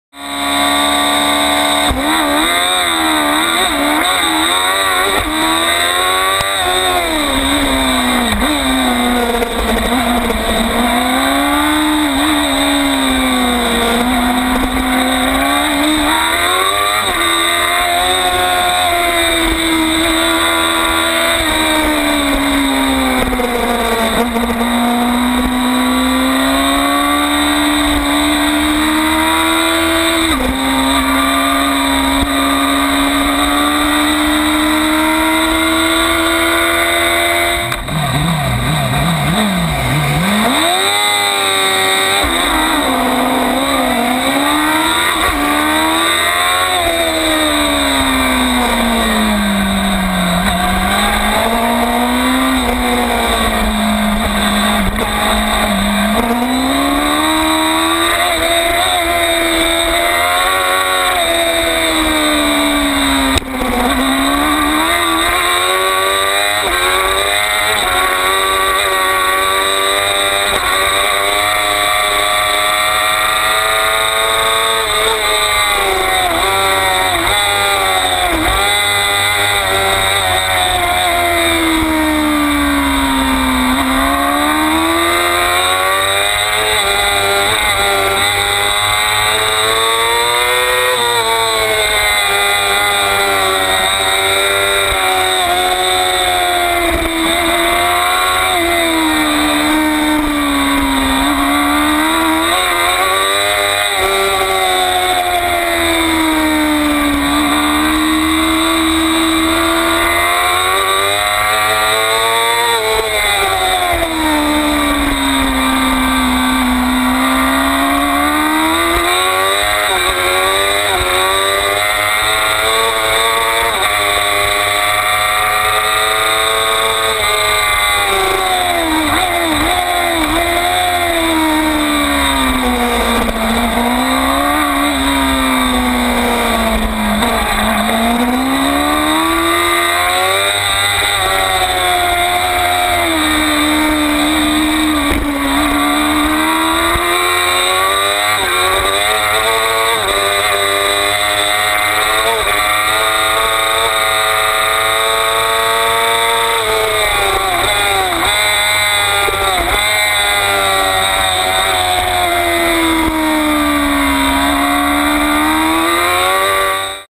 Ryan Harper Hitting the Ground
Ryan Harper crashing on the start of race 3 at MWGP.
Watch Benny Solis and Javelin Broderick on the restart.